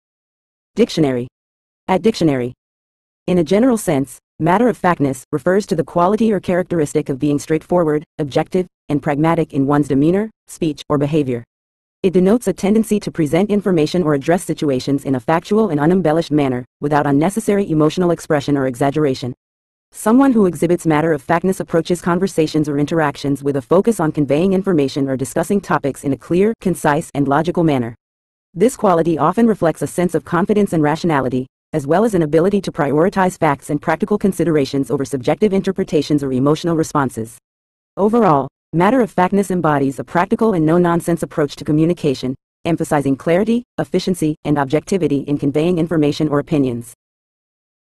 Matter-of-factness Meaning In English
In a general sense, "matter-of-factness" refers to the quality or characteristic of being straightforward, objective, and pragmatic in one's demeanor, speech, or behavior. It denotes a tendency to present information or address situations in a factual and unembellished manner, without unnecessary emotional expression or exaggeration. Someone who exhibits matter-of-factness approaches conversations or interactions with a focus on conveying information or discussing topics in a clear, concise, and logical manner. This quality often reflects a sense of confidence and rationality, as well as an ability to prioritize facts and practical considerations over subjective interpretations or emotional responses. Overall, matter-of-factness embodies a practical and no-nonsense approach to communication, emphasizing clarity, efficiency, and objectivity in conveying information or opinions.